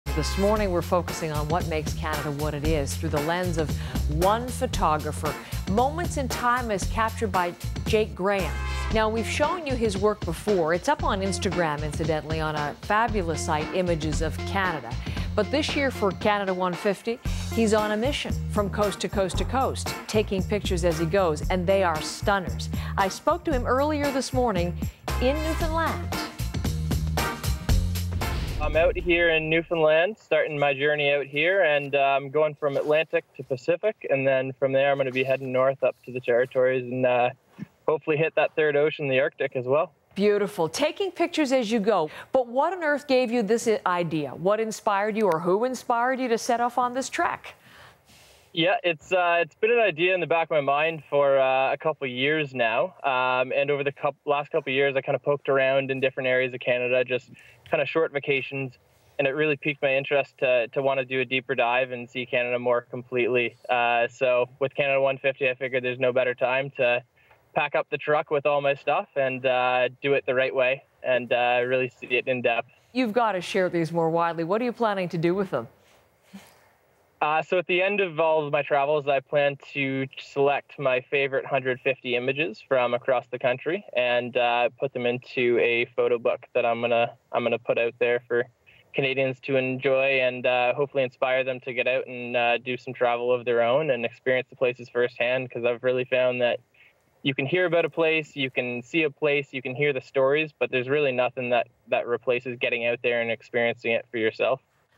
Capturing Canada with 150 stunning photos
Jake Graham is on a coast-to-coast mission, snapping pictures for a photo book to celebrate Canada's sesquicentennial